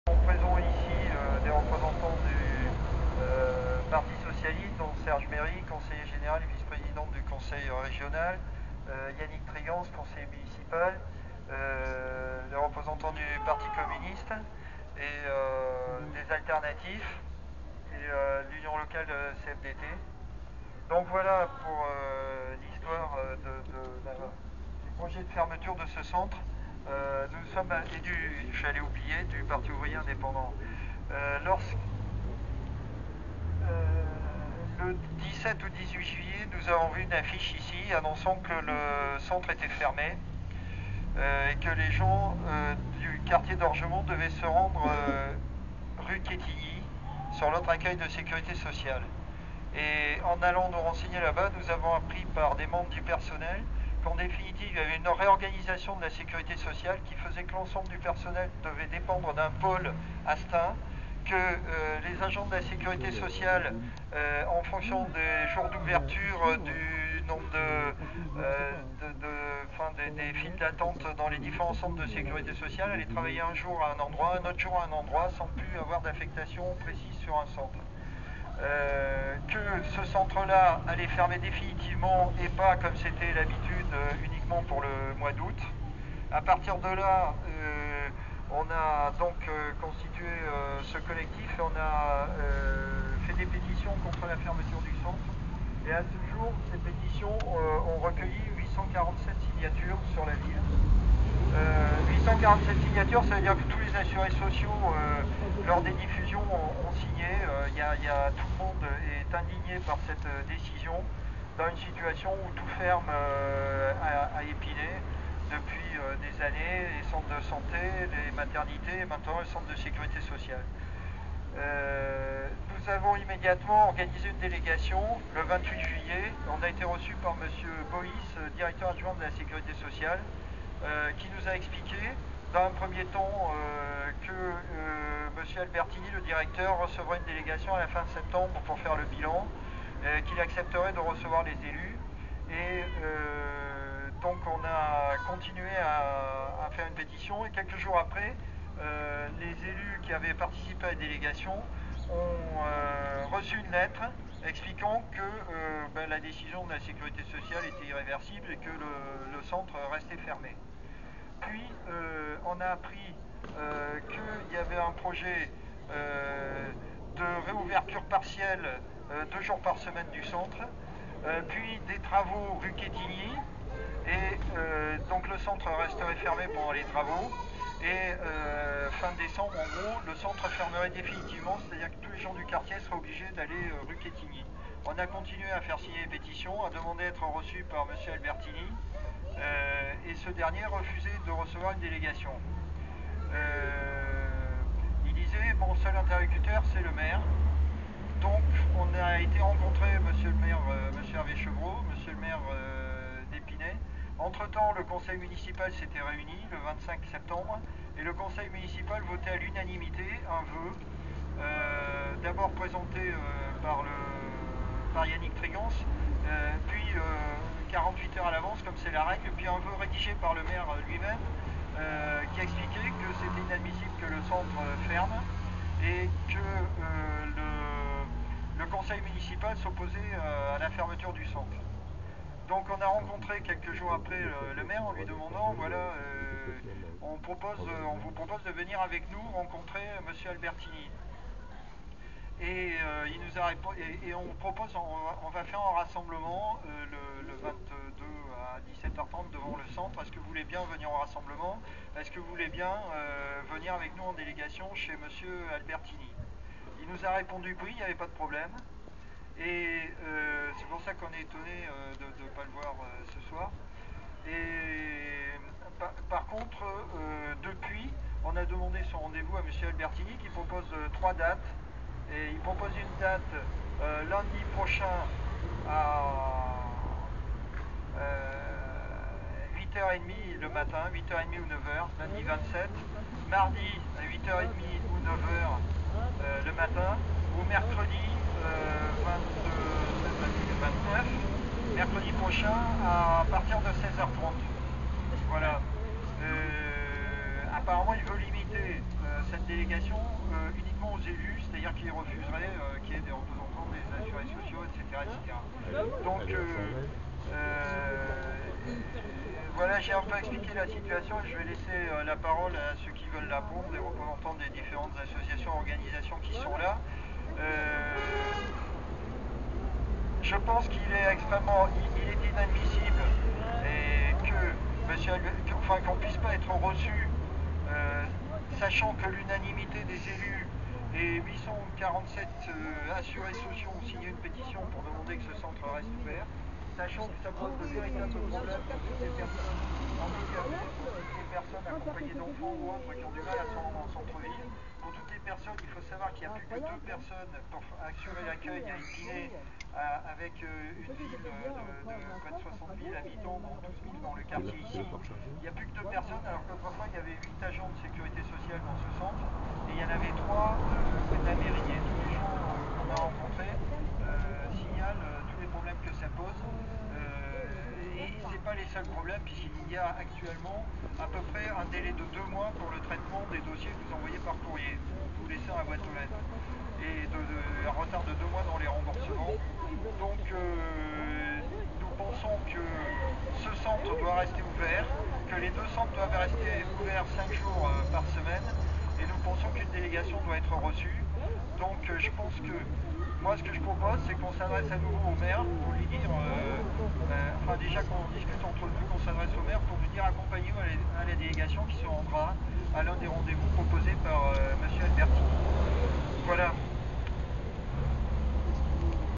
MOBILISATION CONTRE LA FERMETURE DU CENTRE SECU D'ORGEMONT
Rassemblement de Spinassiens du 22 octobre 2008 pour le maintien du Centre de Sécurité Sociale dans le quartier d'Orgemont à Epinay sur Seine (93800)